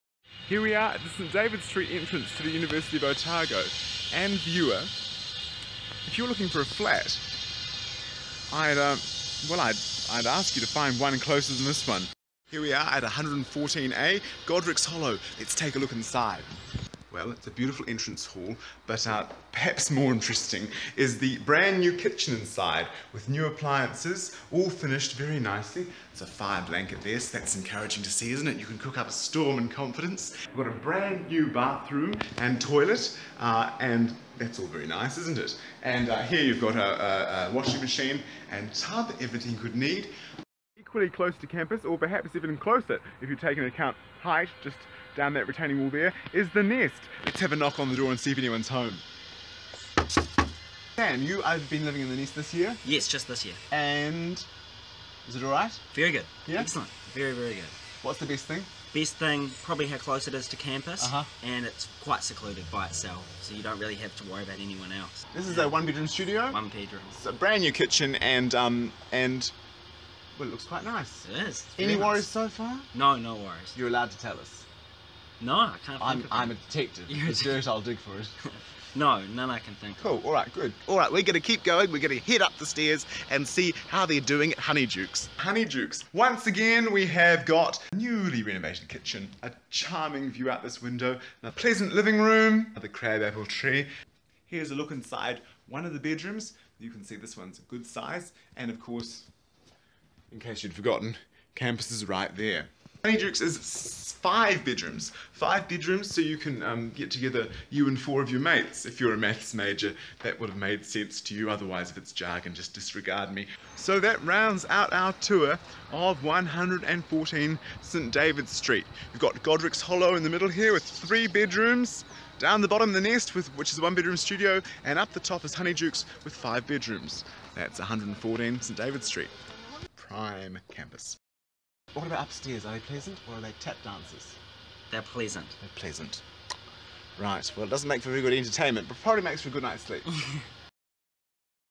Three of the best student flats at 114 St David Street, Dunedin, on the University of Otago grounds
These flats must have to be the best on campus (actually on the University of Otago grounds!), literally seconds away from the Clock Tower building and neighbouring University lecture theatres. In a very sunny location with great views, and homely grounds, these recently renovated flats (Jan 2016) come with all new mod-cons: unlimited high speed fibre internet, 51" TV, SkyTV deal, and new furniture and appliances.